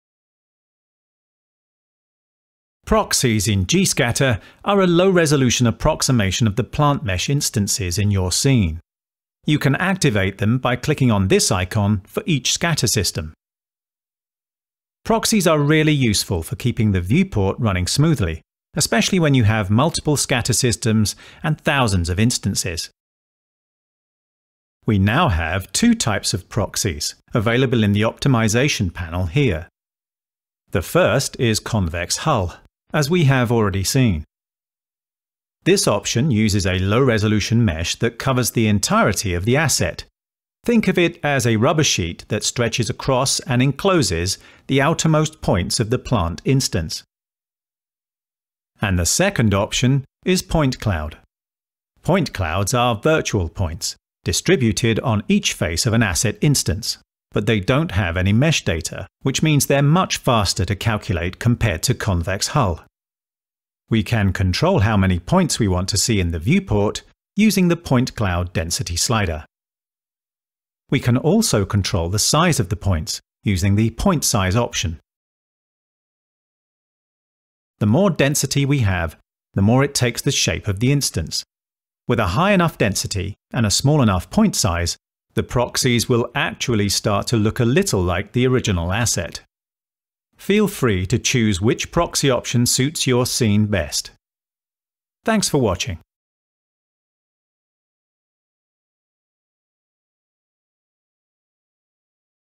Gscatter Features - Proxies
You can now choose between two types of proxies in Gscatter: Convex Hull and Point Cloud. Leverage their power to help keep your Blender scene running smoothly.